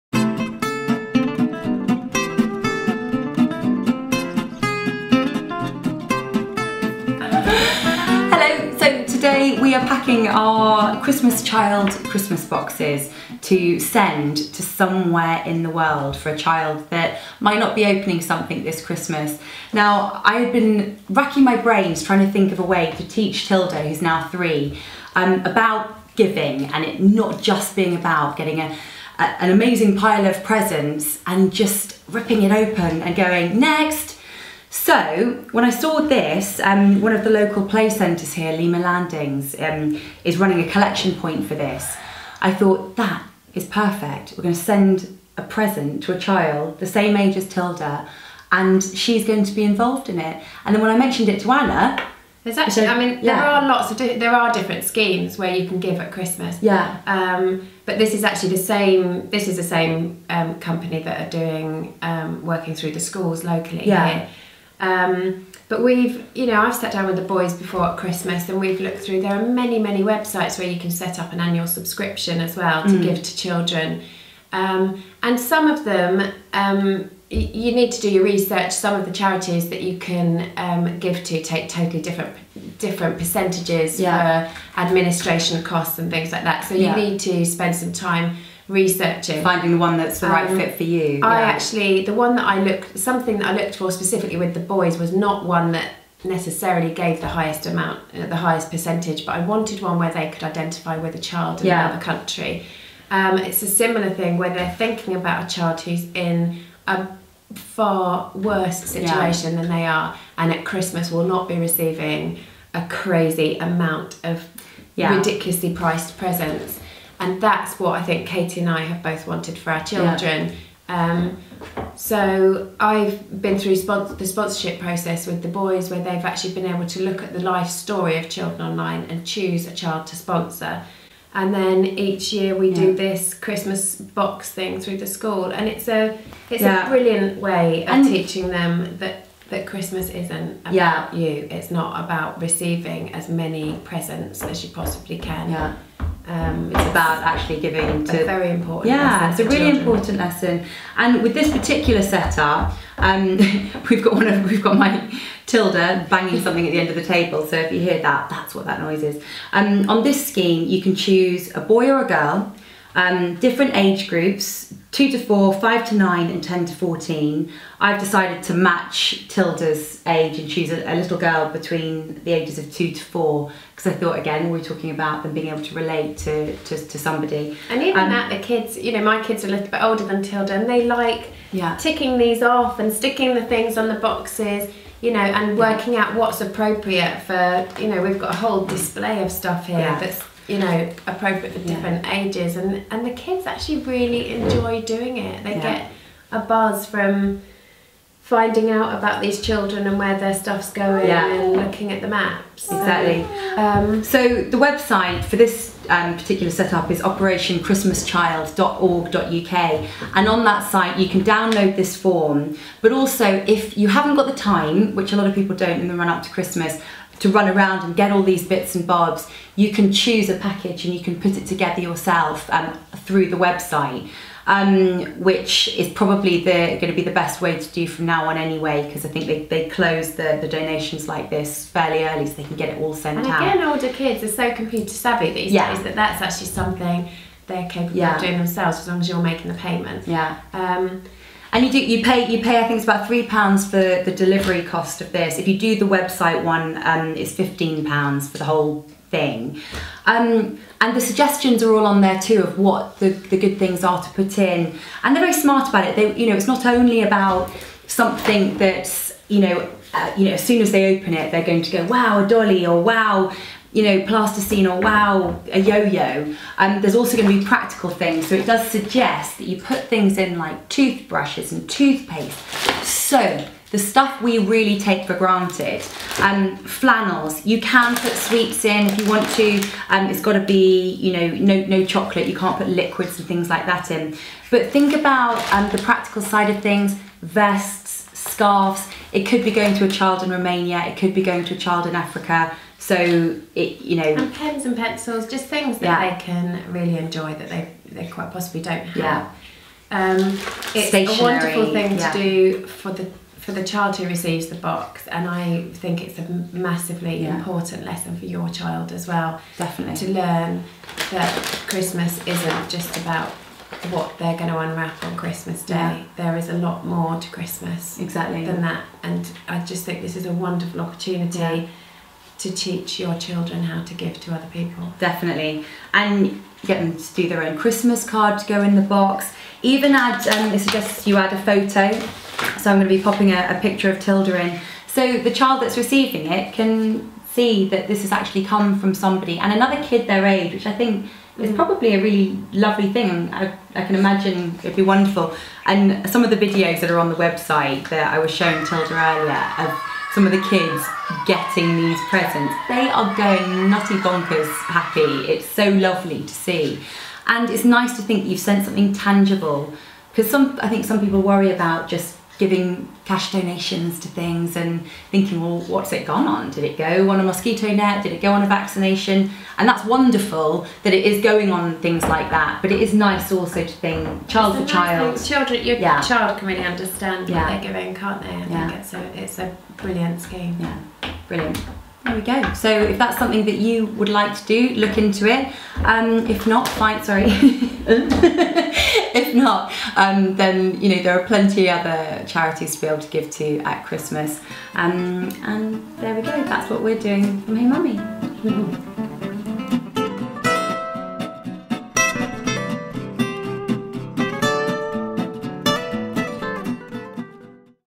Operation Christmas Child
There's no better time to teach your kids about giving than Christmas so Katy and Anna are telling you their plans for sending a shoebox packed full of pressies to a child in need with Operation Christmas Child.